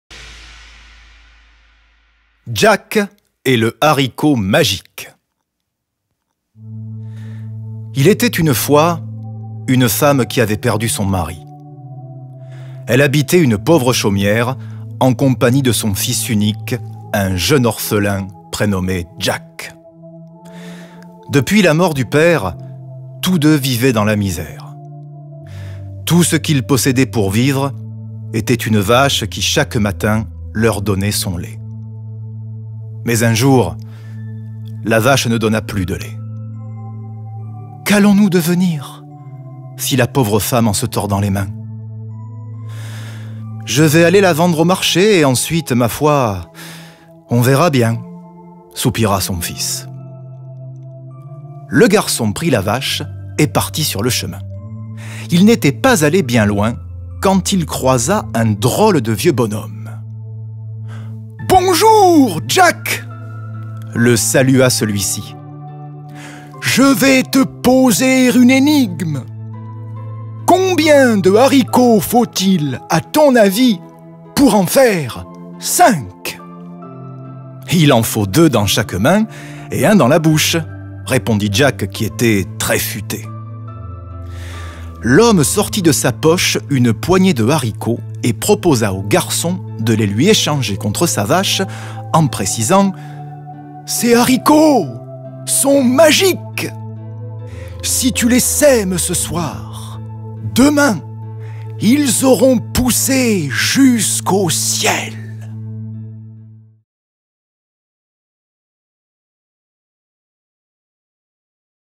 JACK ET LE HARICOT MAGIQUE - Vincent Fernandel raconte
Le livre-CD "Contes classiques racontés par Vincent Fernandel", en librairie le 15 octobre !
En EXCLU, Vincent Fernandel nous interprète un extrait en studio.